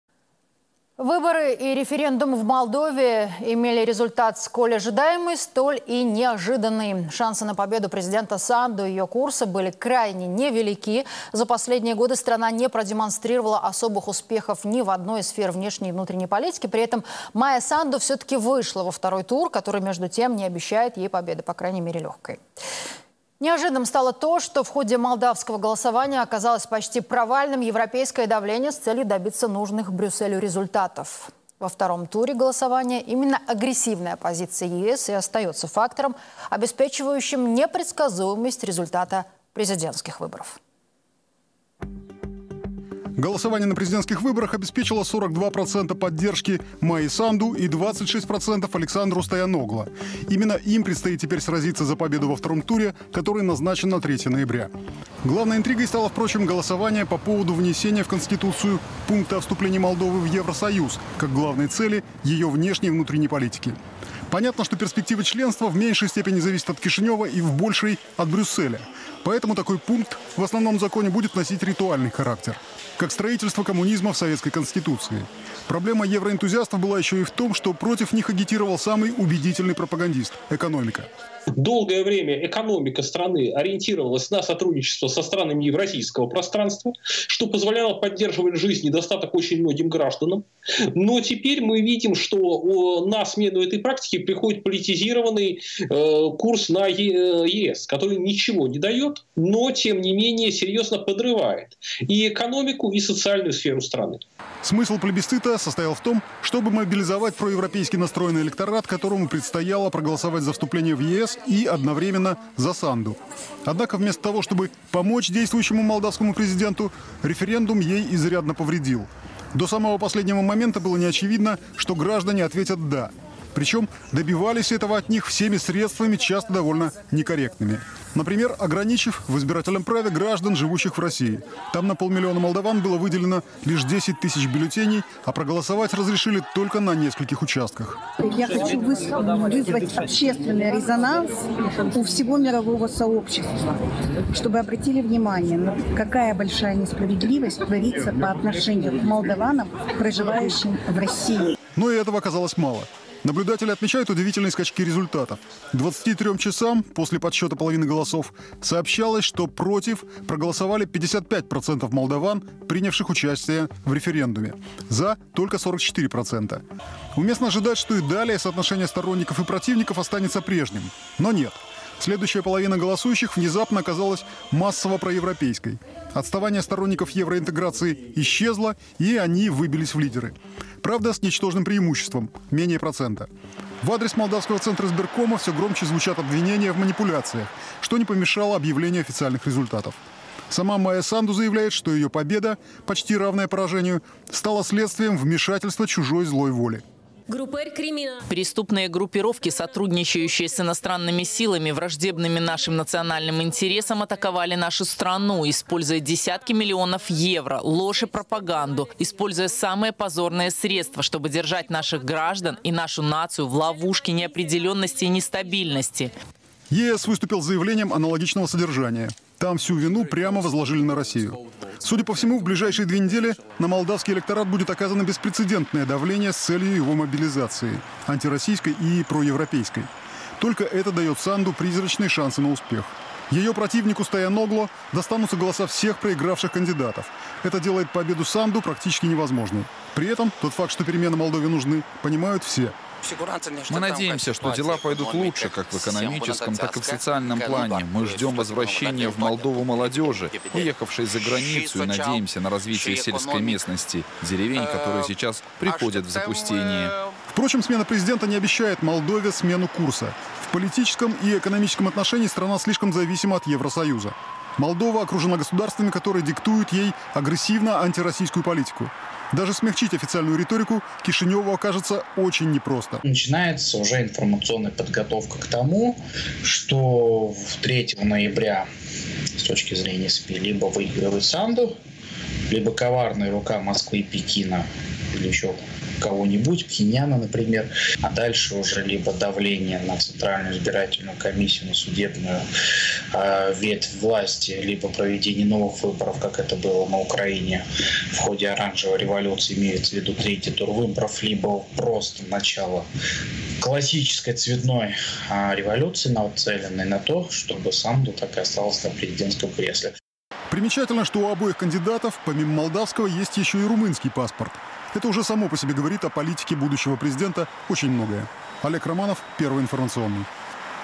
Выборы и референдум в Молдове. О европейском давлении и итогах первого тура.
Выборы и референдум в Молдове имели результат сколь ожидаемый, столь и неожиданный.

Шансы на победу президента Санду и ее курса были крайне невелики. За последние годы страна не продемонстрировала особых успехов ни в одной из сфер внешней и внутренней политики.

☝🏼При этом Майя Санду все-таки вышла во второй тур, который, между тем, не обещает ей легкой победы.

Неожиданным стало то, что в ходе молдавского голосования оказалось почти провальным европейское давление с целью добиться нужных Брюсселю результатов.

Во втором туре голосования именно агрессивная позиция ЕС и остаётся фактором, обеспечивающим непредсказуемость результата президентских выборов.